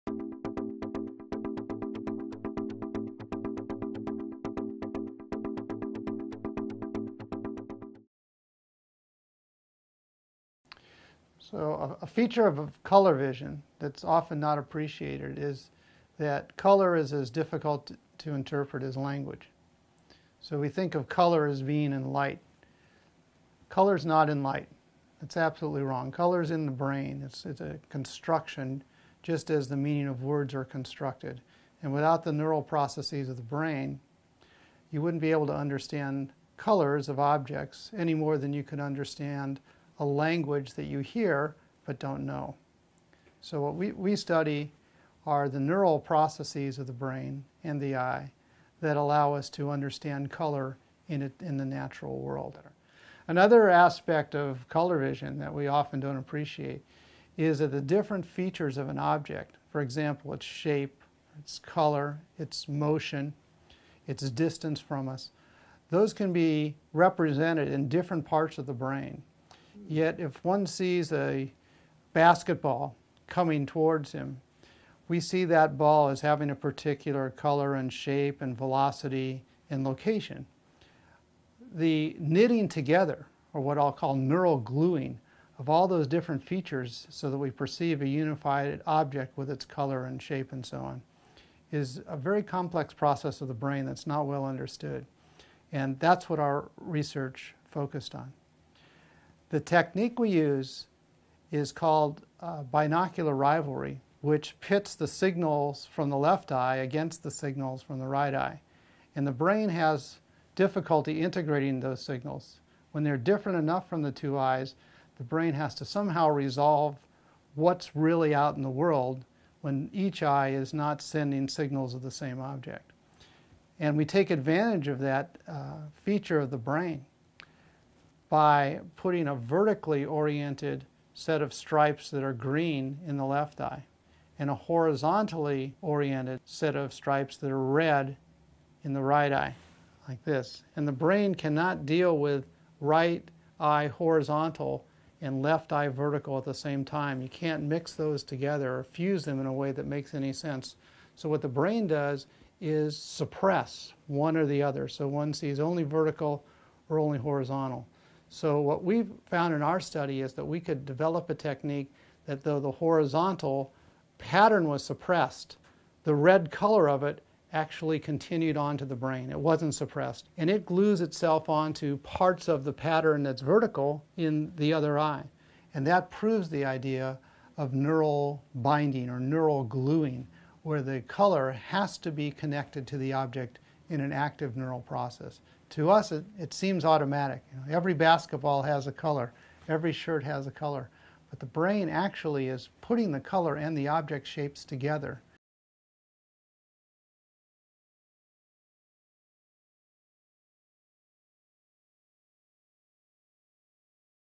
How the brain sees color
Research at the University of Chicago has demonstrated, for the first time, that instead of disappearing along with the lost object, the color latches onto a region of some other object in viewa finding that reveals a new basic property of sight.